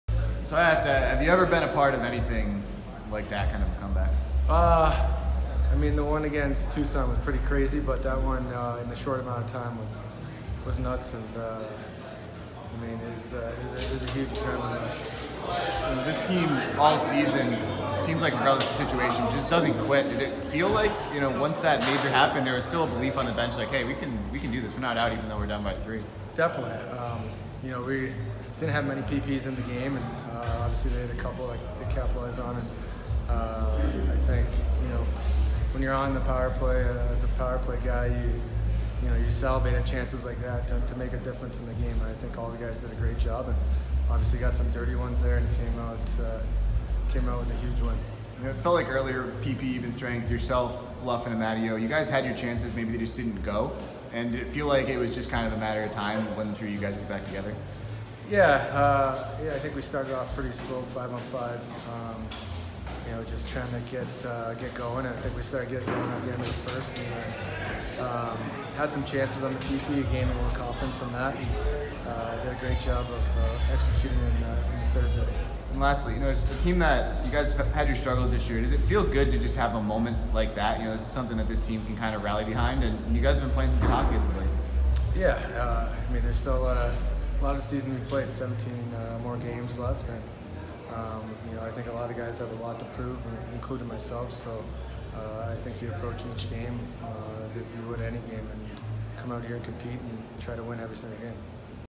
Matt Moulson - 3/2
Matt Moulson after scoring the game-winning goal in a 5-4 overtime win over San Jose in March 2 in Ontario.